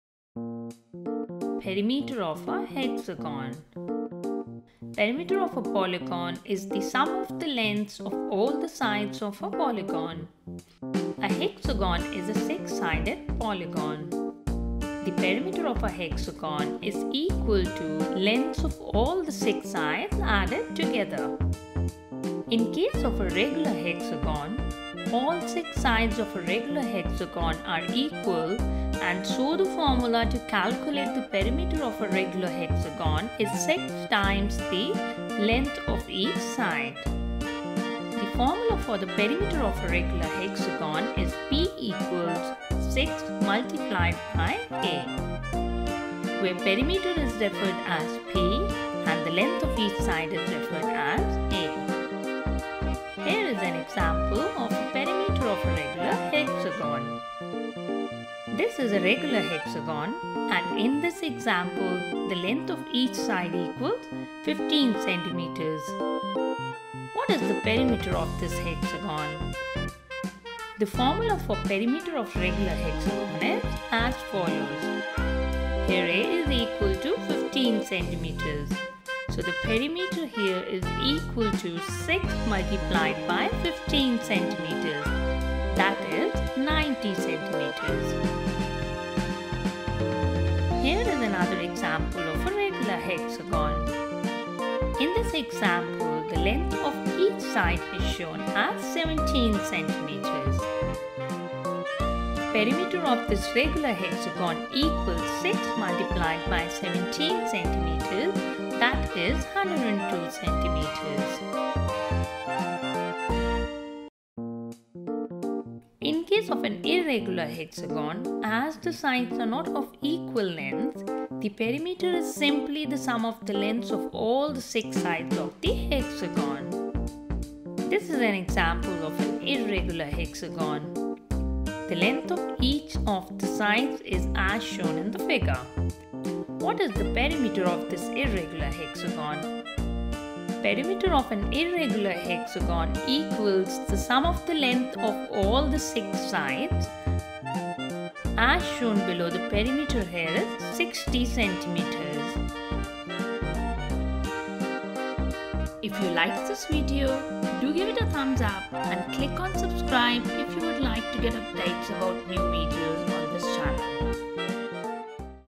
Perimeter of Hexagon, Perimeter for Regular and Irregular Hexagon with Formula & Examples
This is an educational video about Perimeter of a Hexagon. This video has formula and examples related to Perimeter of Regular and Irregular Hexagons.

Perimeter of a polygon is the sum of the lengths of all the sides of a polygon.

A hexagon is a 6 sided polygon.

The perimeter of a hexagon is equal to lengths of all the 6 sides added together

In case of a regular hexagon, all 6 sides of a regular hexagon are equal and so the formula to calculate the perimeter of a regular hexagon is 6 times the (length of each side)

The Formula for the Perimeter of a regular hexagon is P equals 6 multiplied by a (P = 6a)
Where perimeter is referred as “P” and the length of each side is referred as “a”.

Here is an example of a perimeter of a regular hexagon

This is a regular hexagon and in this example the length of each side equals 15cm
What is the perimeter of this hexagon?

The Formula for Perimeter of regular hexagon is as follows
Here a is equal to 15cm

So the perimeter here is equal to 6 multiplied by 15cm that is 90 cm

Here is another example of a regular hexagon. In this example the length of each side is shown as 17cm.

Perimeter of this regular hexagon equals 6 multiplied by 17cm that is 102cm

In case of an irregular hexagon, as the sides are not of equal lengths, the perimeter is simply the sum of the lengths of all the 6 sides of the hexagon.

This is an example of an irregular hexagon, the length of each of the sides is as shown in the figure. What is the perimeter of this irregular hexagon?